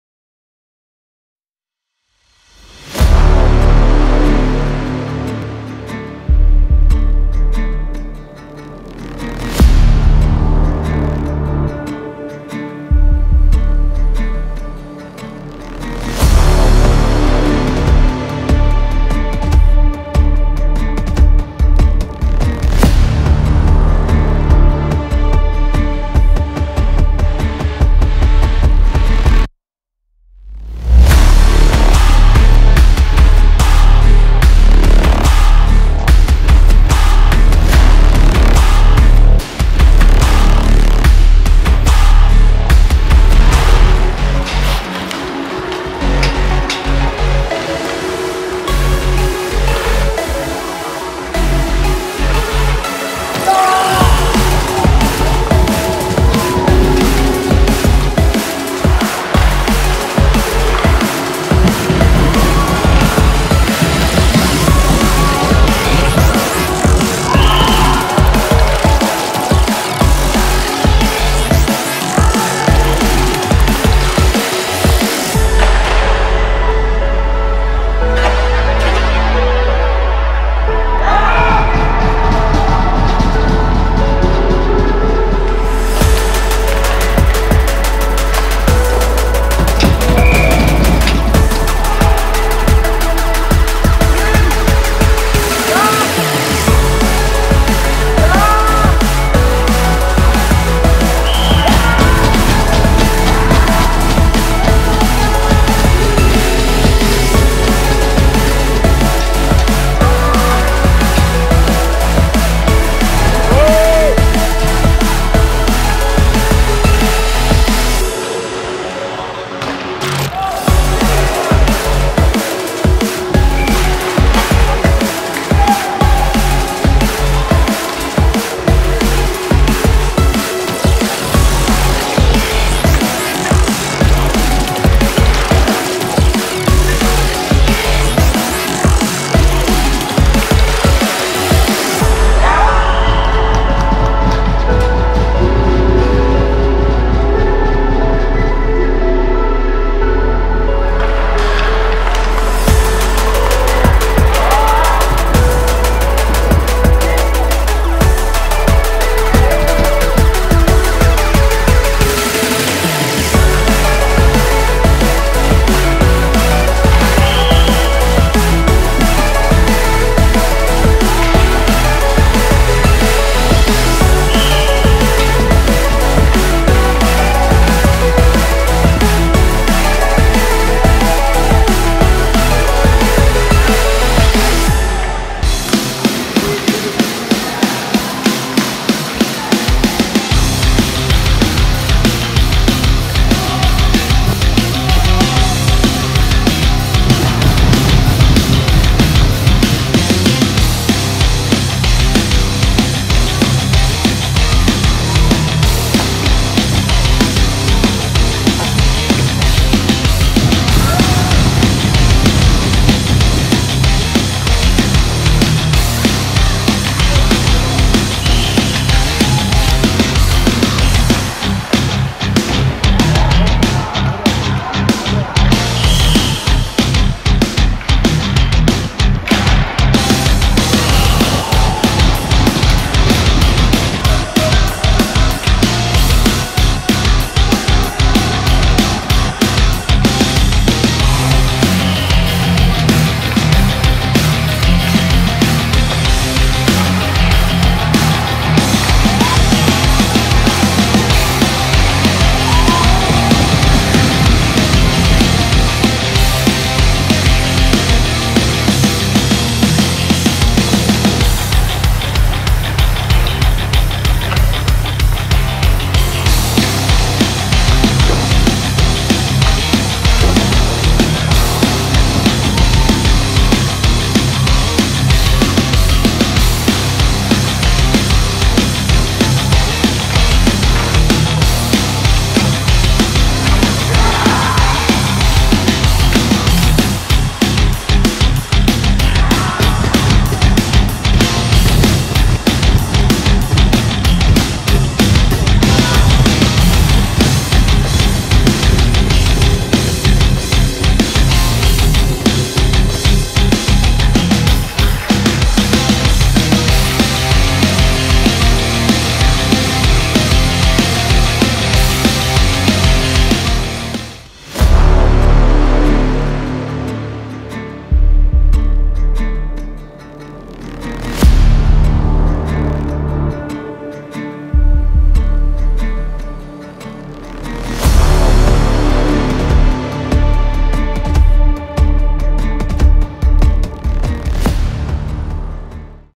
U15 Meisterrunde 22/23: EHC 80 Nürnberg - Augsburger EV am 19.11.2022: Highlights
erstes Heimspiel in der neuen Runde